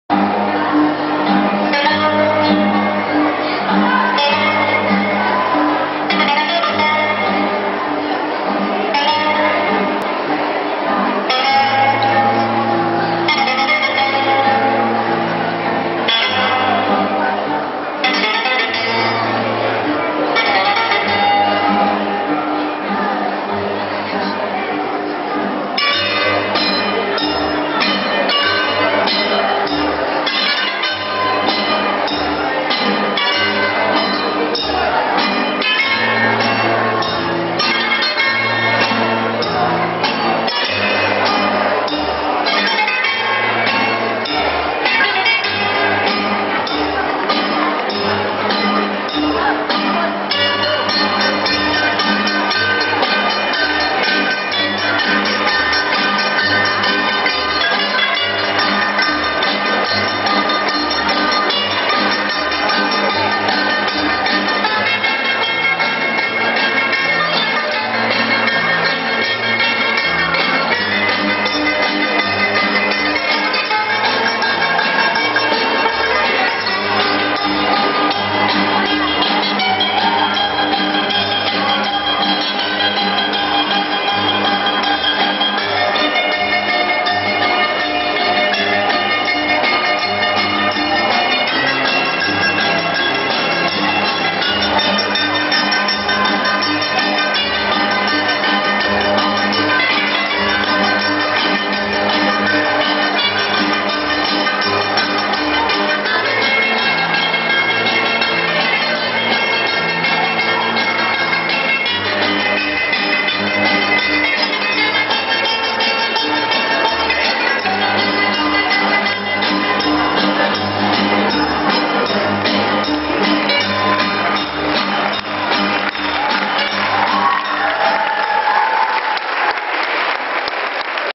A 3.b osztály tánca
2012. február 10. farsangi Projektnap, Eötvös József ÁMK